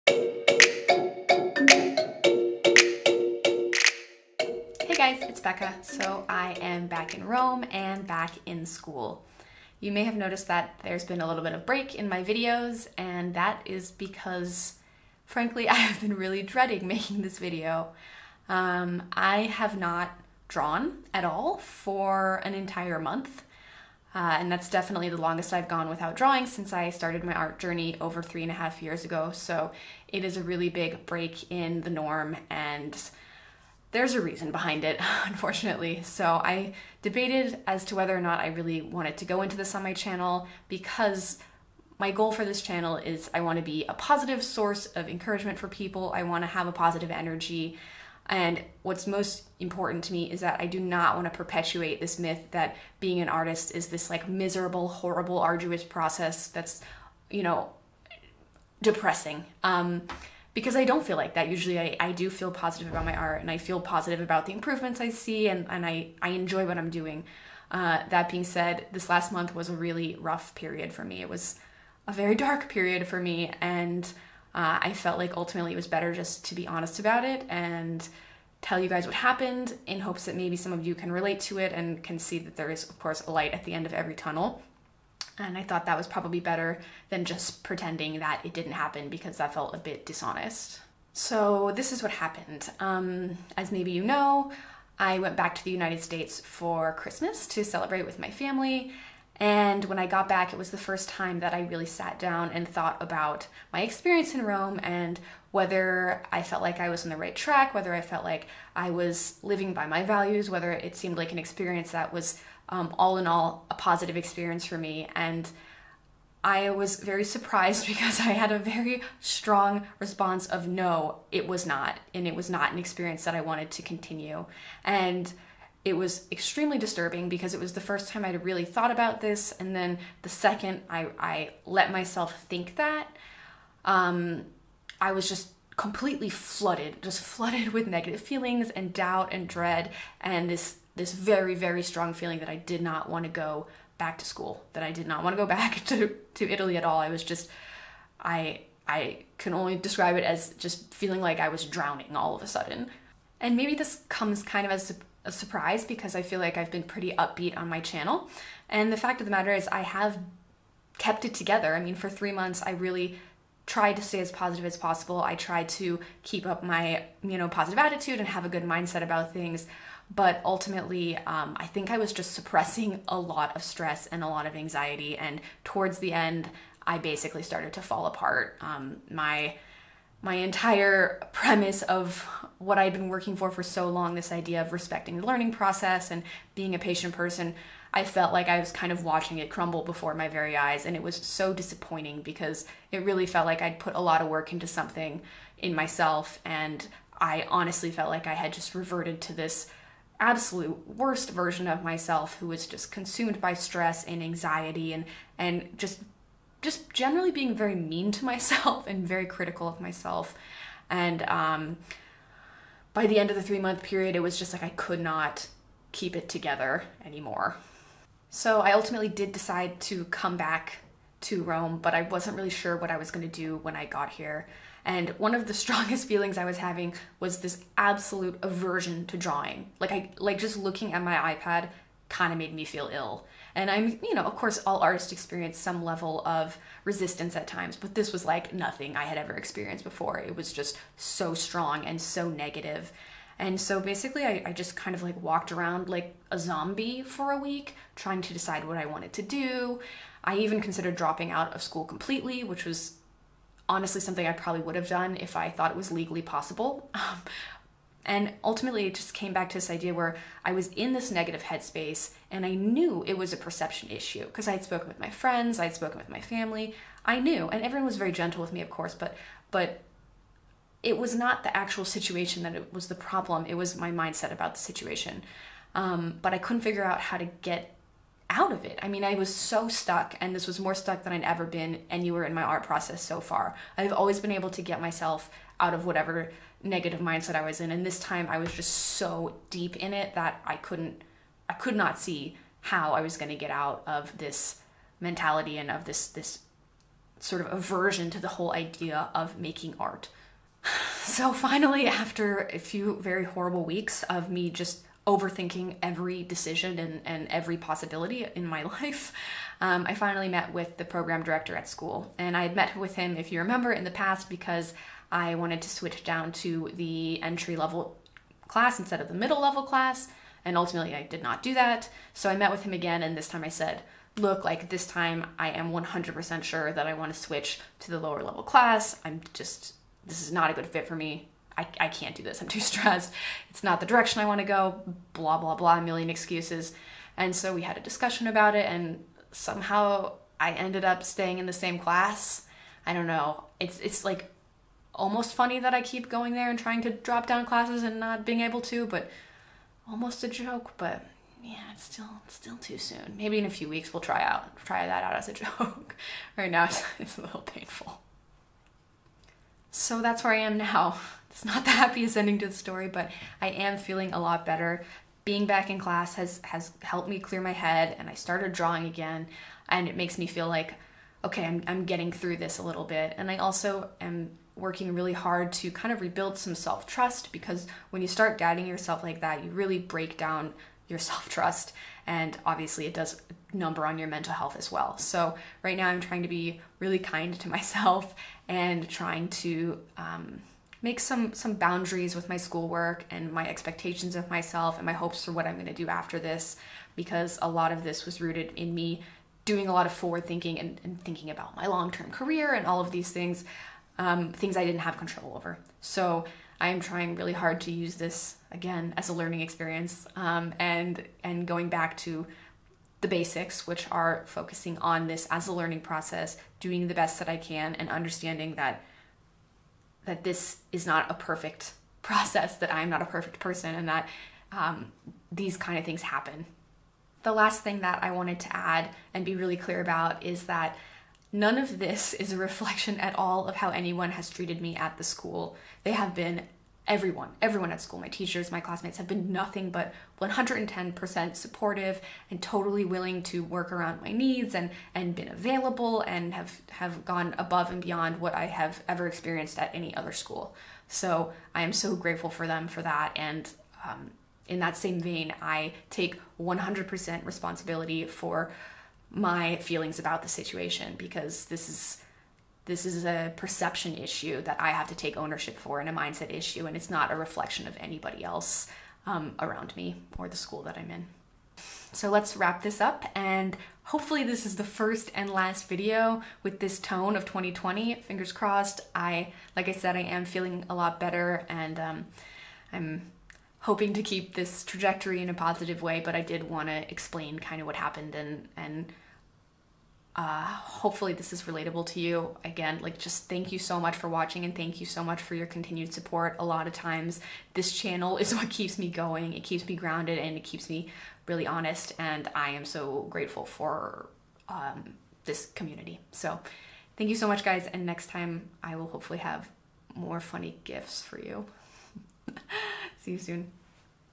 Playing the Long Game (Surviving Artist's Block)
Welp this is easily the least funny video I have ever made. Sorry in advance for the lack of gifs and jokes. The last few weeks have been a struggle, but I'm finally starting to feel like myself again. More like a pigeon stumbling out of a dumpster than a Phoenix rising from the ashes, but it's a start. :)